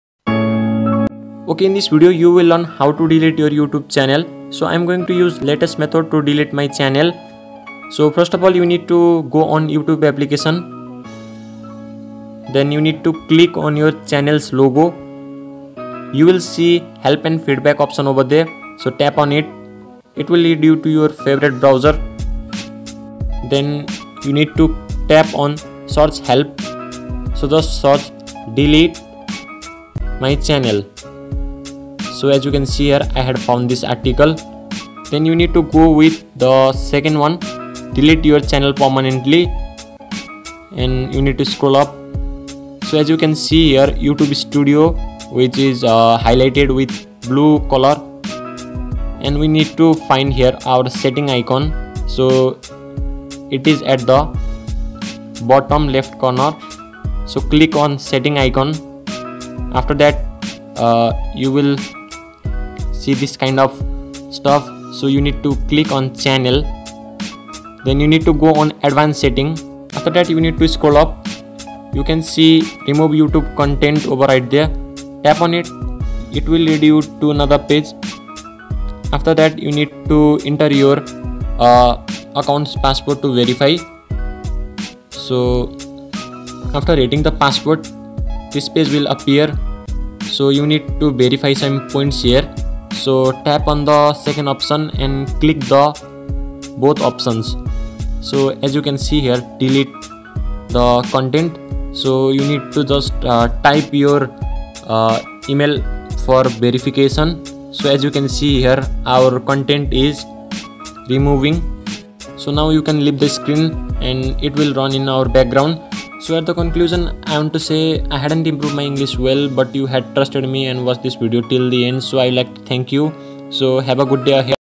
How to delete a youtube channel | Delete youtube channel (2022) Updated
Do you want to delete a YouTube Channel? Deleting a channel is permanent and it will delete all posts and comments alongside all your videos.

So in this video you will learn "how you can delete your YouTube channel" permanently on android phone or mobile. This is the latest method of 2022 with which you can permanently delete or remove youtube channel or account.

Chapters
0:00 Intro
0:10 Go to Youtube App
0:30 Find article
0:45 Go to Youtube Studio
1:05 Go to setting
1:17 Verification
1:33 Delete your Channel
1:56 Conclusion

Video Covers:
how to delete youtube channel 2022
how to delete a youtube channel